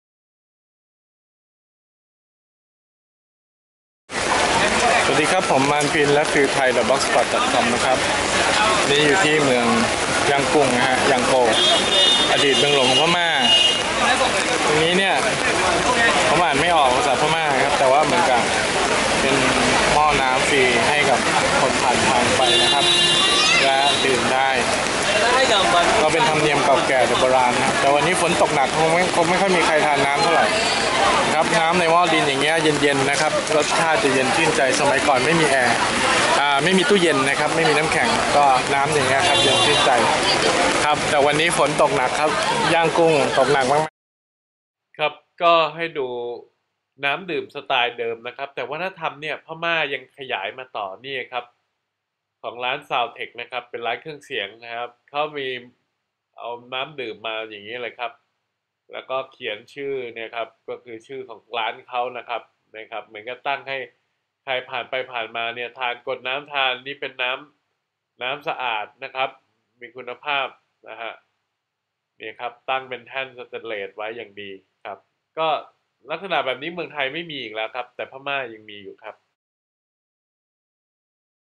วัฒนธรรมน้ำของพม่า
ที่พม่าหรือเมียนมาร์ก็มีวัฒนธรรมการต้อนรับผู้มาเยือนหรือคนผ่านทางไปมาด้วยน้ำในหม้อดินเผาเย็นเย็น คนสมัยก่อนไม่มีตู้เย็น น้ำที่ตัดจากภาชนะแบบนี้ จะมีอุณหภูมิต่ำกว่าน้ำที่ใส่ ในภาชนะแบบอื่น วัฒนธรรมแบบนี้ก็ยังตกมาถึงรุ่นปัจจุบัน จริงแล้วที่พม่าเขายังนิยมปฏิบัติกันมากกว่าเมืองไทยย้อนหลัง ดูภาพแบบนี้แทบจะไม่ได้แล้วยกเว้นตามจังหวัดห่างไกลจริงเท่านั้น